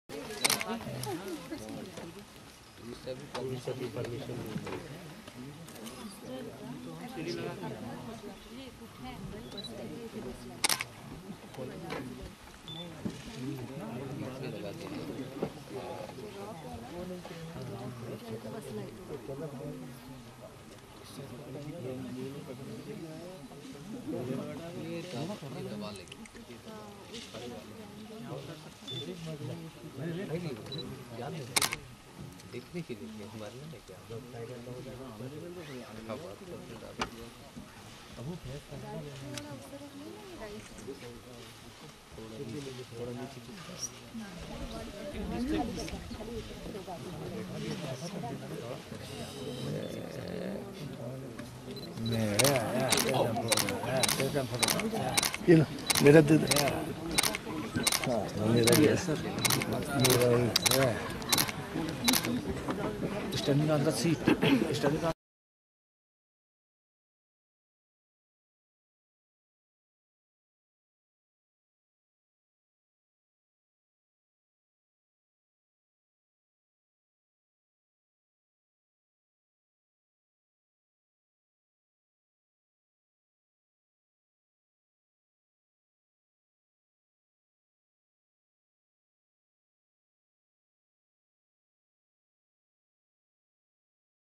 Our First Wild Tiger - Clip 2- Kanha - India
- After crossing the road 'Number 7 Male' drank for 5 minutes!